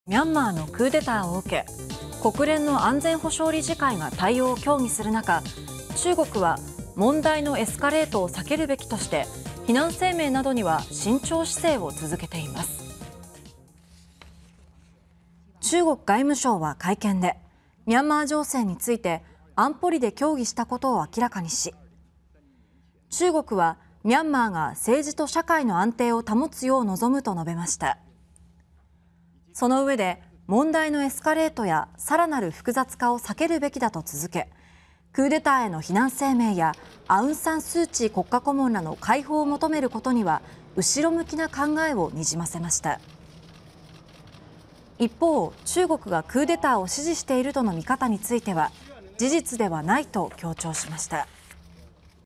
スー・チー氏解放要求や非難声明に後ろ向きな中国(2021年2月3日)
ミャンマーのクーデターを受け、国連の安全保障理事会が対応を協議するなか、中国は「問題のエスカレートを避けるべき」として非難声明などには慎重姿勢を続けています。

　中国外務省は会見で、ミャンマー情勢について安保理で協議したことを明らかにし、「中国はミャンマーが政治と社会の安定を保つよう望む」と述べました。

　そのうえで、「問題のエスカレートやさらなる複雑化を避けるべきだ」と続け、クーデターへの非難声明やアウン・サン・スー・チー国家顧問らの解放を求めることには後ろ向きな考えをにじませました。

　一方、中国がクーデターを支持しているとの見方については、「事実ではない」と強調しました。